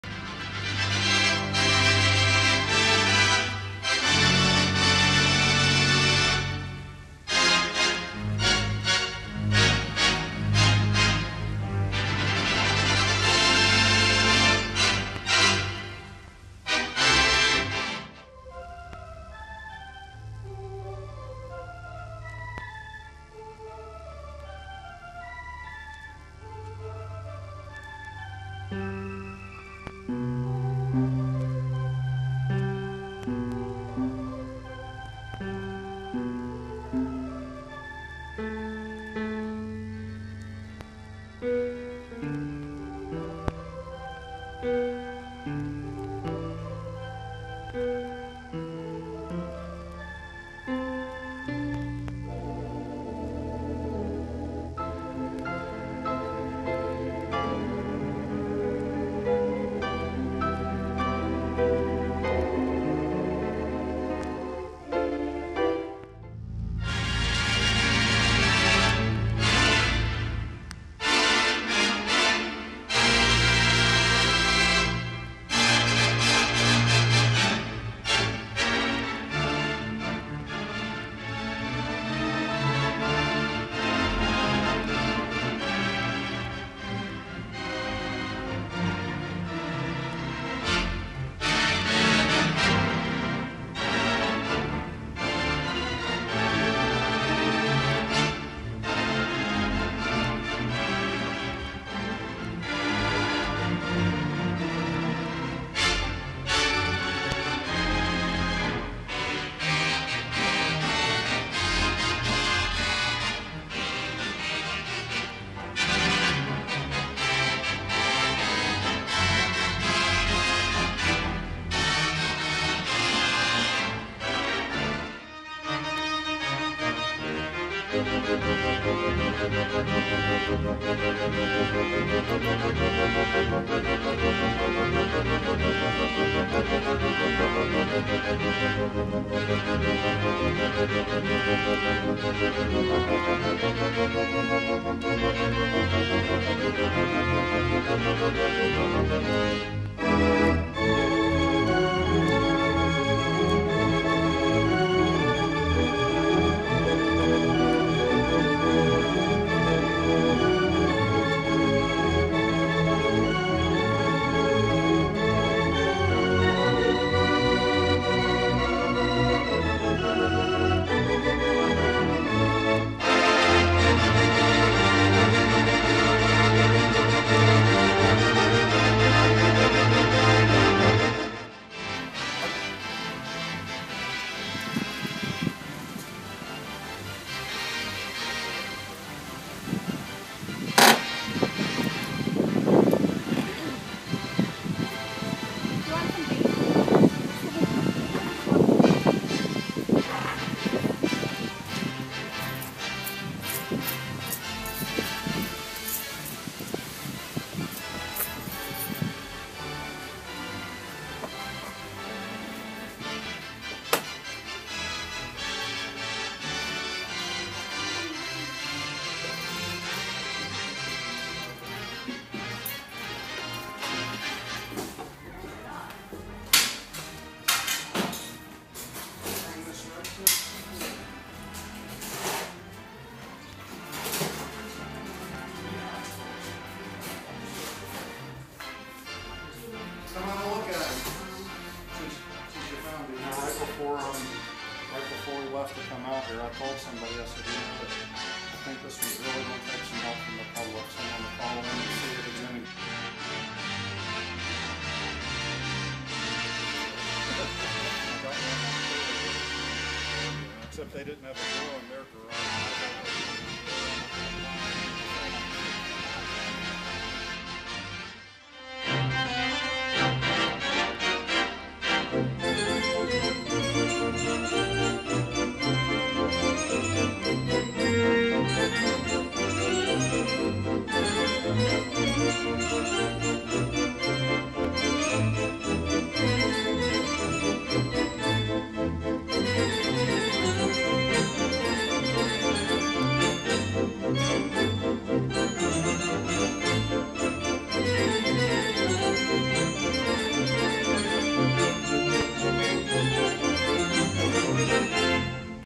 The Great Birthday Escape!
The Great Birthday Escape! with Maky the Lemur Center mascot as Avion the Lemur.
A dramatic reenactment of the lemur escape on May 24, 2013, as reported in the WRAL evening news, with actual footage of the lemur's rescue.

On Friday, May 24, Avior, a free-ranging red ruffed lemur, celebrated his 5th birthday. As a birthday present, he decided to leave the Lemur Center for a stroll through a leafy Durham neighborhood. Avior was recovered later that afternoon when a local homeowner successfully trapped the lemur in his garage and called the Duke Lemur Center. Avior was a little scared but healthy and unharmed thankfully.

Local news WRAL covered the story in the evening news, which is available on their WRAL also got the lemur's name wrong; they called him Avion.

Music from archive.org, Wurlizter Organ selections from George Wright and George Blackmore.